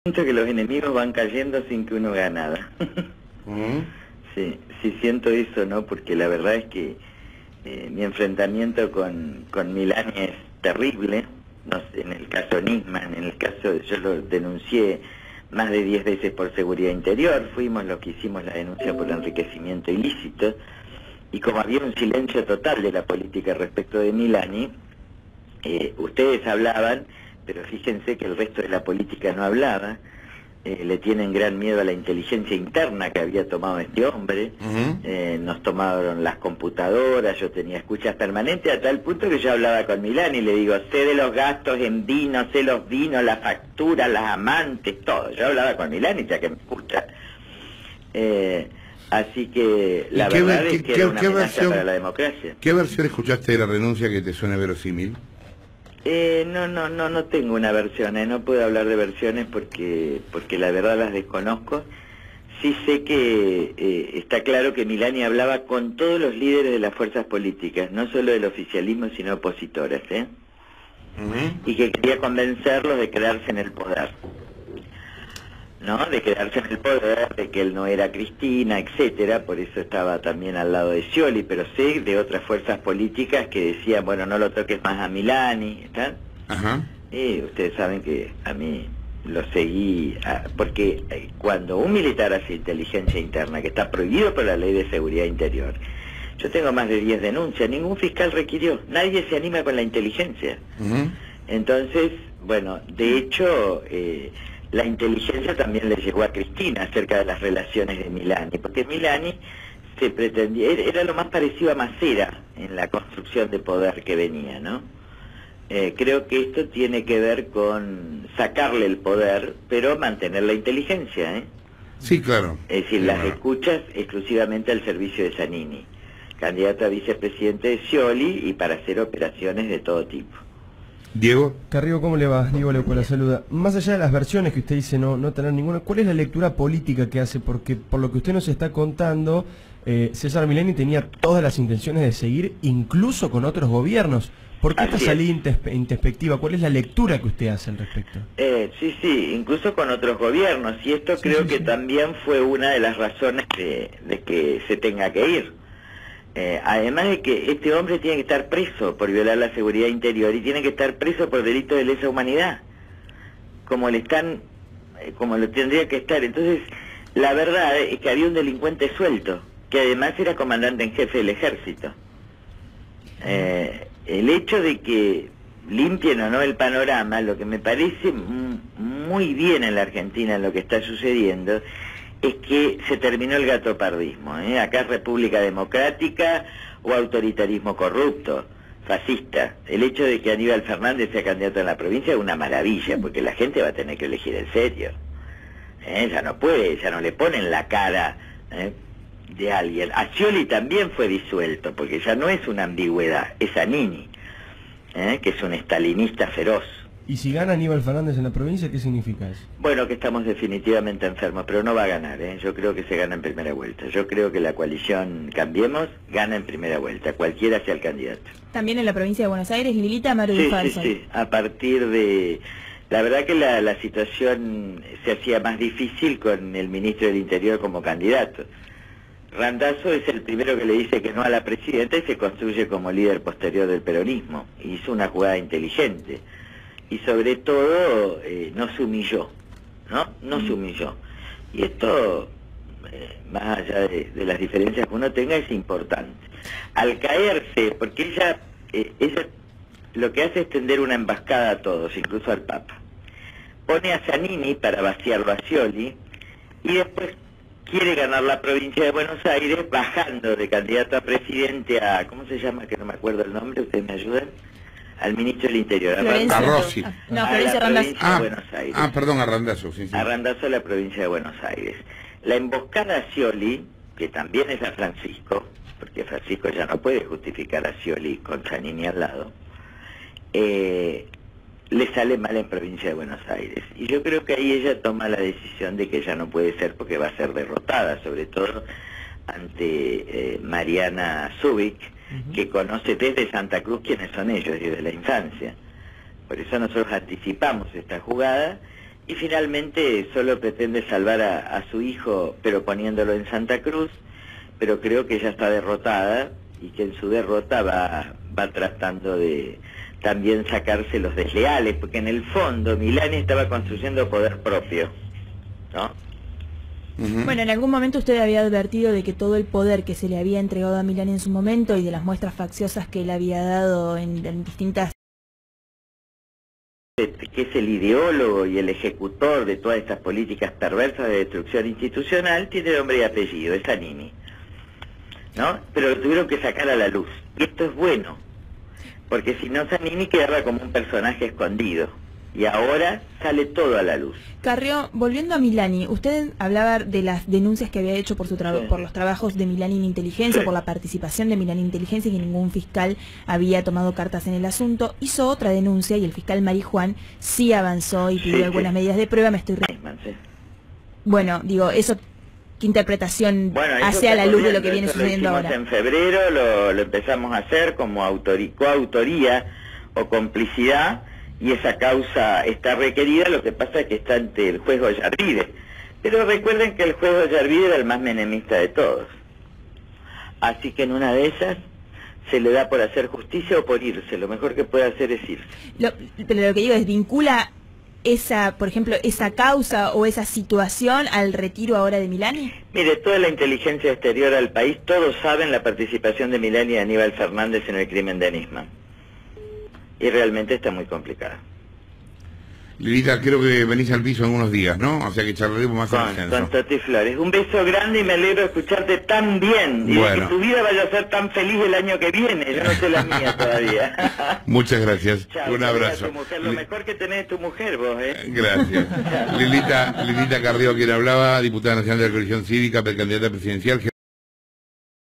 Había un delincuente suelto que además era Comandante en jefe del Ejército. 24/06/2015
Había un delincuente suelto que además era Comandante en jefe del Ejército.
MILANI TIENE QUE ESTAR PRESO POR VIOLAR LA SEGURIDAD INTERIOR Y POR DELITOS DE LESA HUMANIDAD
Esto tiene que ver cuando un militar hace inteligencia interna, que está prohibido por la ley de seguridad interior, yo tengo más de 10 denuncias y ningún fiscal requirió, nadie se anima con la inteligencia.
Creo que esto – (la renuncia)-  tiene que ver en sacarle el poder pero mantener la inteligencia, es decir las escuchas exclusivamente al servicio de Zannini candidato a vicepresidente de Scioli y para hacer operaciones de todo tipo.
El hecho de que limpien o no el panorama lo que me parece muy bien en la Argentina, los que está sucediendo, es que se terminó el gatopardismo. Acá es República Democrática o Autoritarismo Corrupto Fascista; el hecho de que Aníbal Fernández sea candidato en la Provincia es una ¡maravilla! Porque la gente va a tener que elegir en serio.